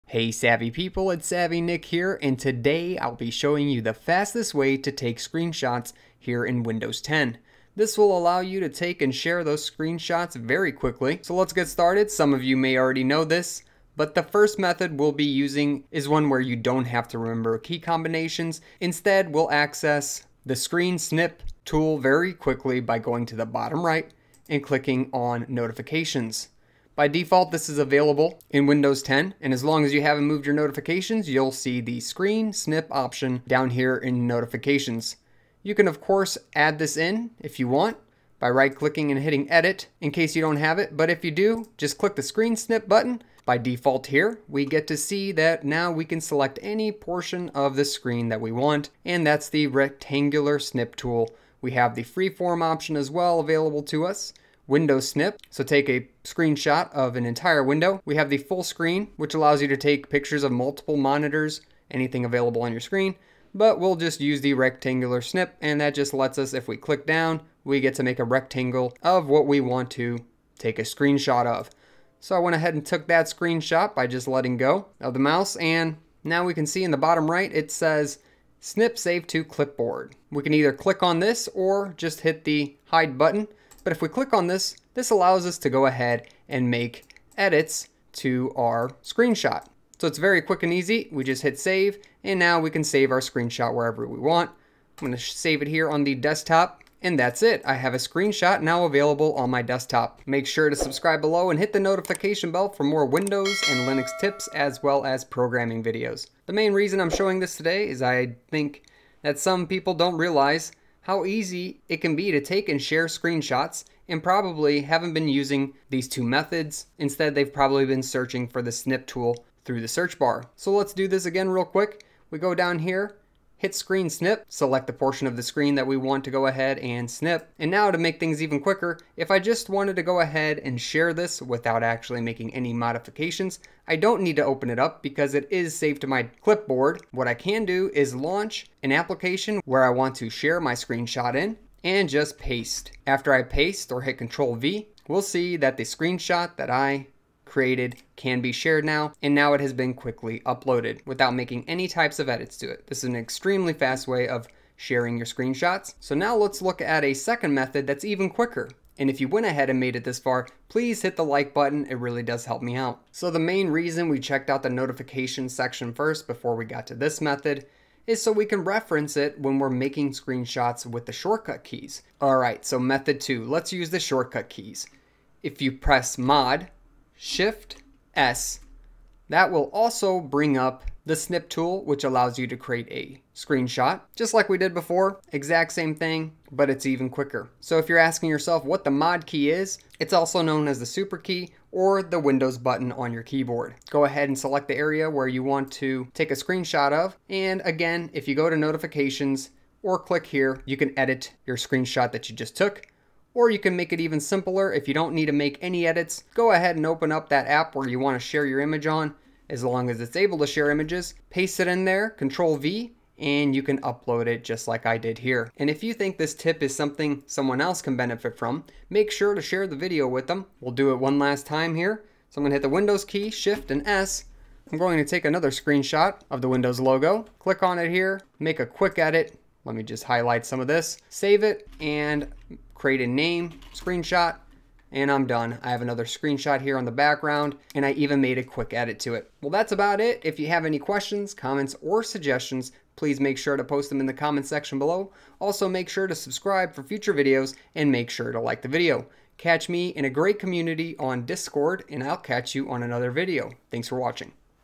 FASTEST WAY - How to Take and Share a Screenshot on Windows 10
How to Take and Share a Screenshot on Windows 10 will show you the FASTEST way to use the Snip Tool to create/take a Screenshot on Windows 10 / PC. Then share it on a program. This works on all Windows 10 computers and other PC's. Make sure you learn the fastest way to take and share screenshots. This is a quick and easy tutorial in 2021. Easily take screenshots on Windows 10 and use paste instead of saving the screenshot!

Using the snip tool in notifications or using the shortcut key combinations are the two fastest ways to take screenshots. Not only do they take quick screenshots, but then you can easily share those screenshots across platforms/programs easily by pasting them into your program without having to save the screenshot on to the computer. Learn how to do this with this video! THE FASTEST WAY to take and share a screenshot on Windows 10 / PC.